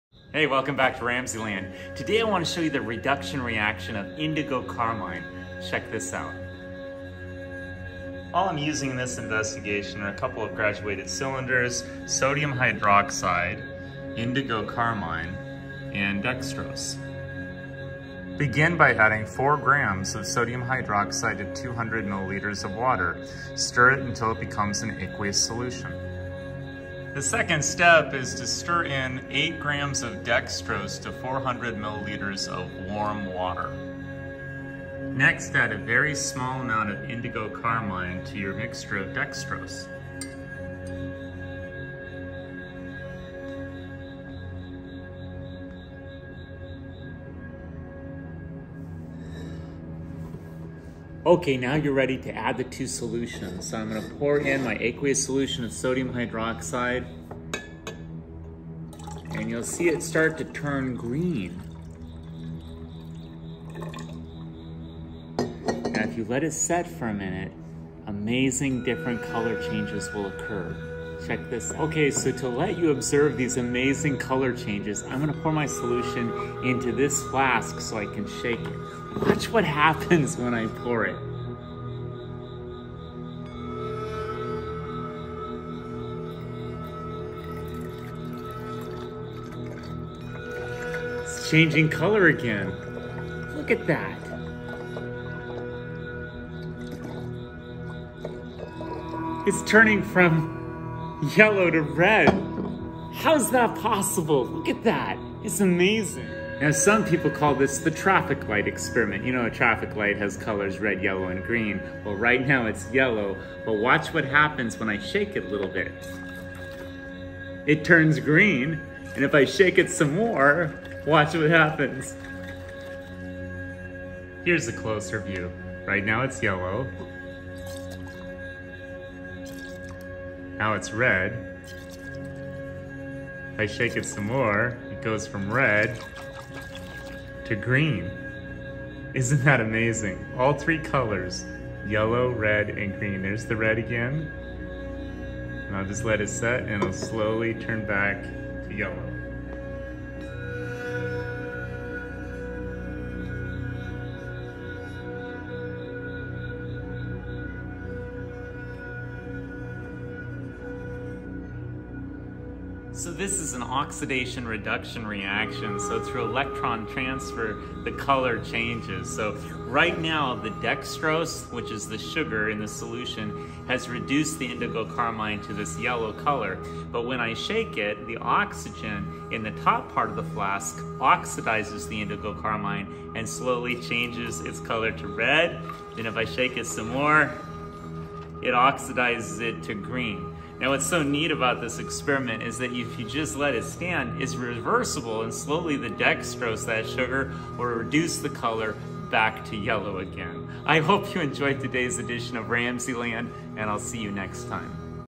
Redox of Indigo Carmine in RamZland!⚗️
Keith Ramsey demonstrates the reduction and oxidation of indigo carmine in this popular “traffic light” experiment. Bridgeport Elementary School of the MSD of Wayne Township is a STEM certified public school.